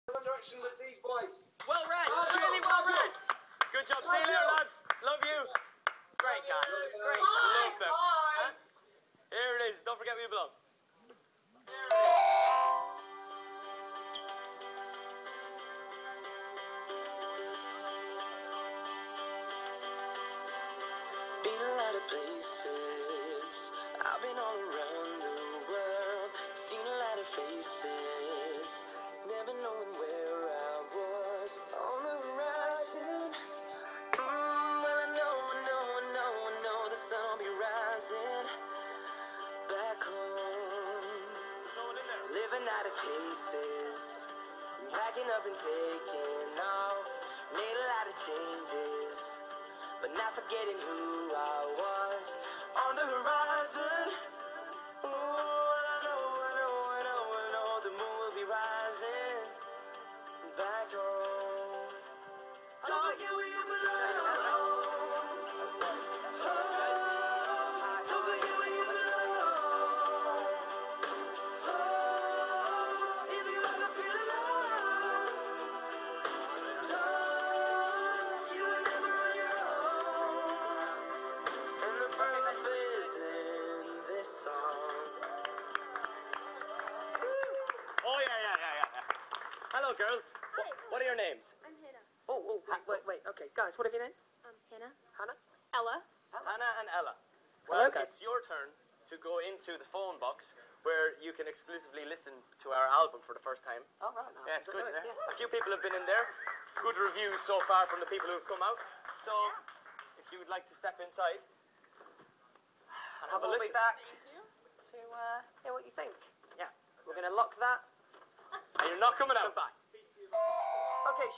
Louis and Niall dancing on 1D Day
Niall & Louis dancing to their new song!